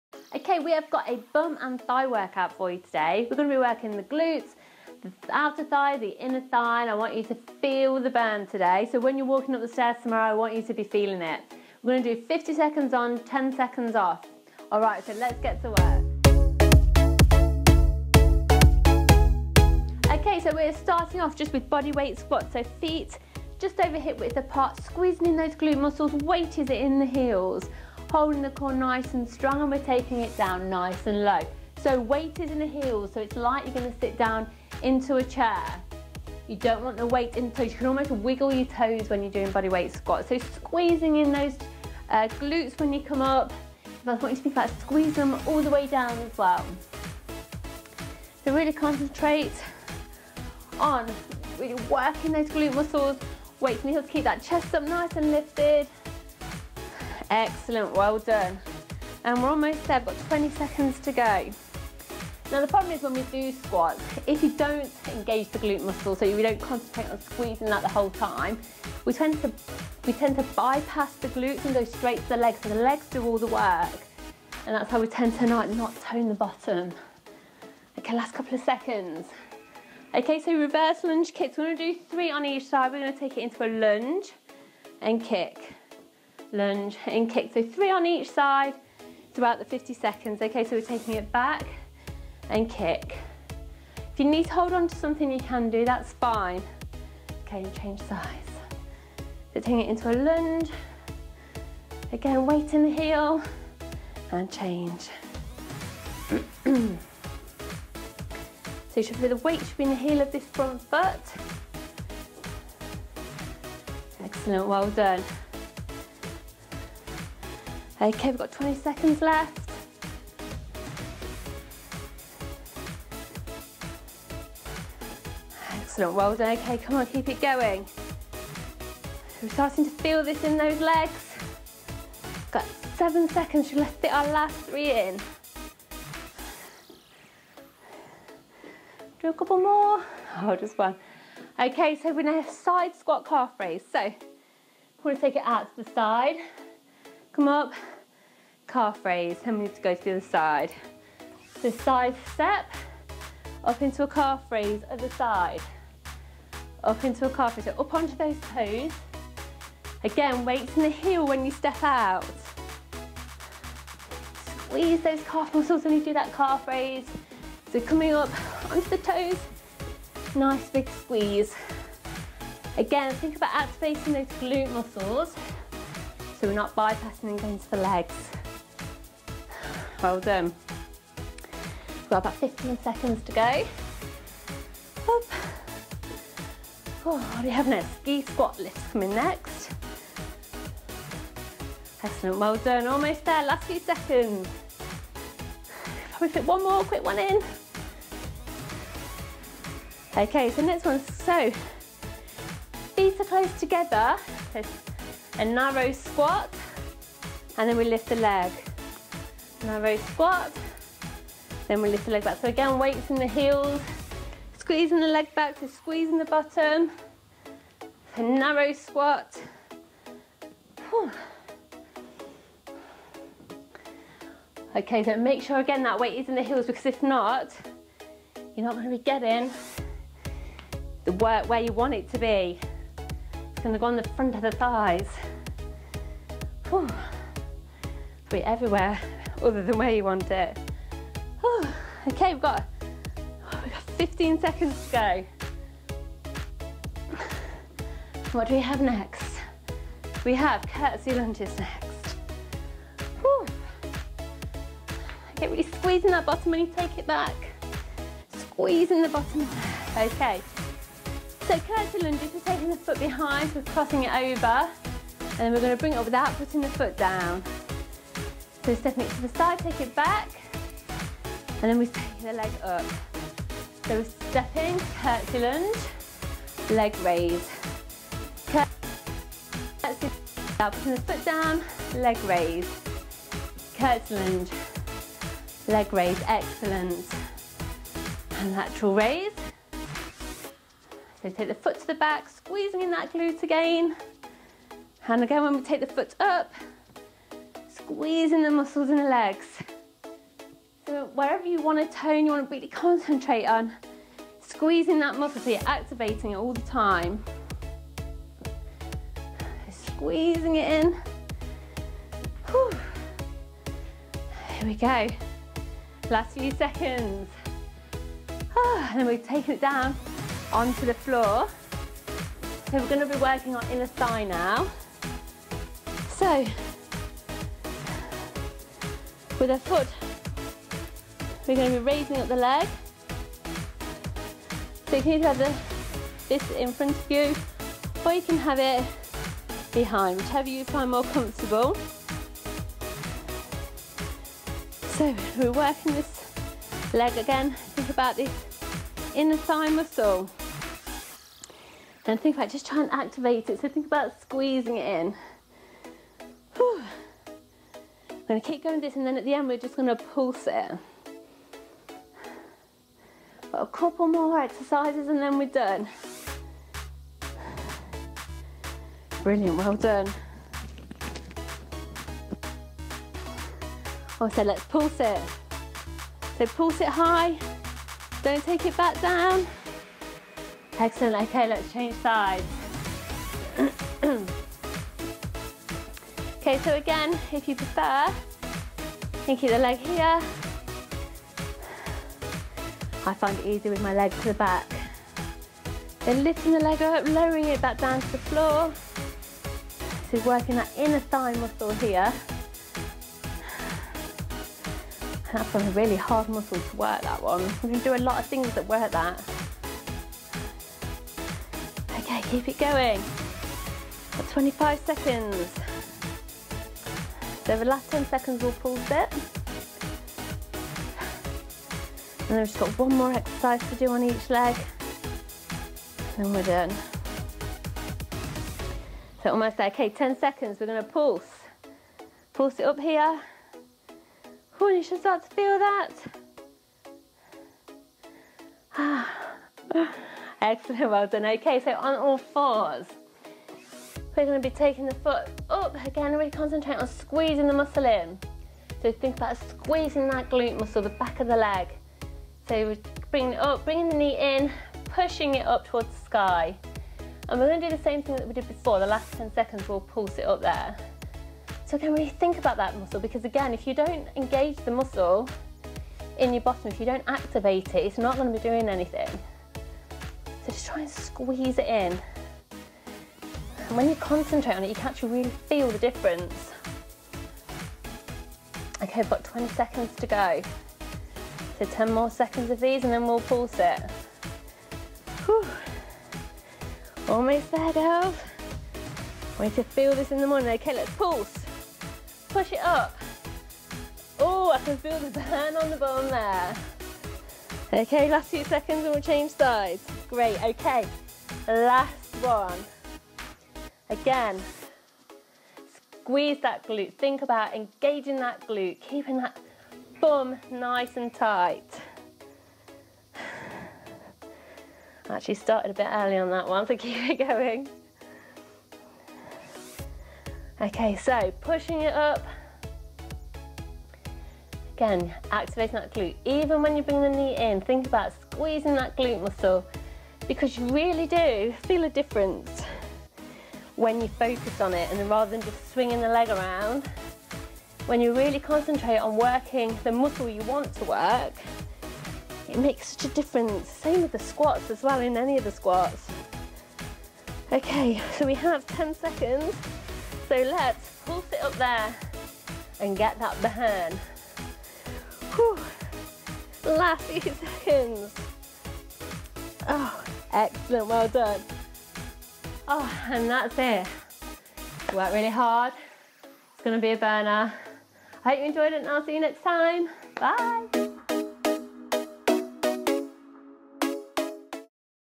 Exercises to tone your bum!
Lets burn that butt!  This workouts help to lift, tone and strengthen the bottom and thighs.